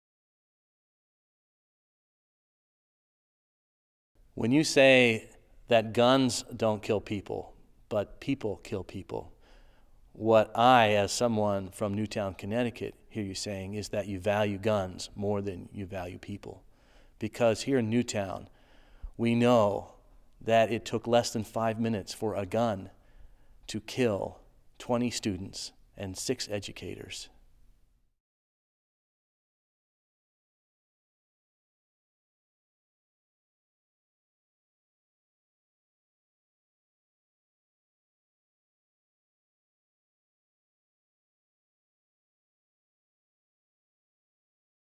When You Say "Guns don't kill people"
In this video from the United Church of Christ, listen to what many people hear when you say "Guns don't kill people, people kill people."